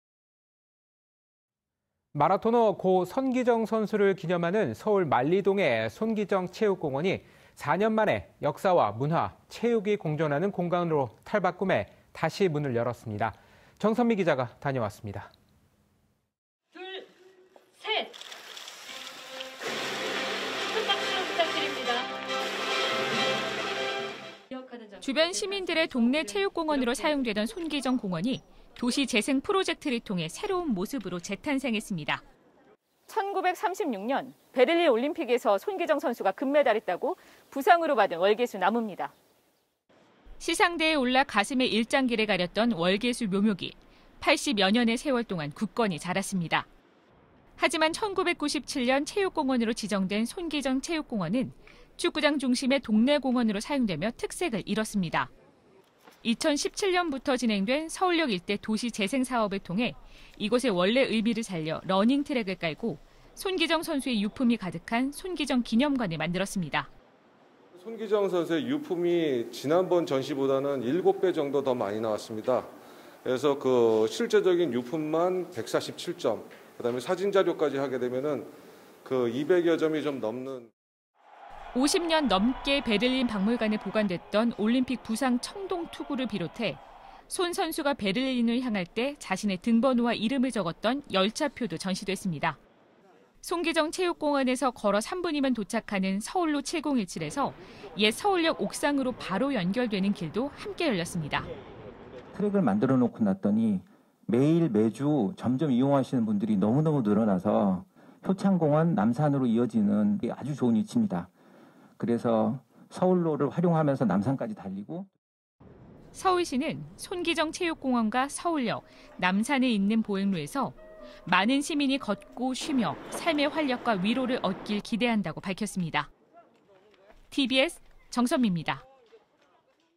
손기정 체육공원 역사·문화·체육 공존 공간으로 탈바꿈
대한민국에 처음으로 올림픽 금메달을 건넨 선수죠,

마라토너 고 손기정 선수를 기념하는 서울 만리동의 '손기정 체육공원'이

4년 만에 역사와 문화·체육이 공존하는 공간으로 탈바꿈해 오늘 다시 문을 열었습니다.

정선미 기자가 보도합니다.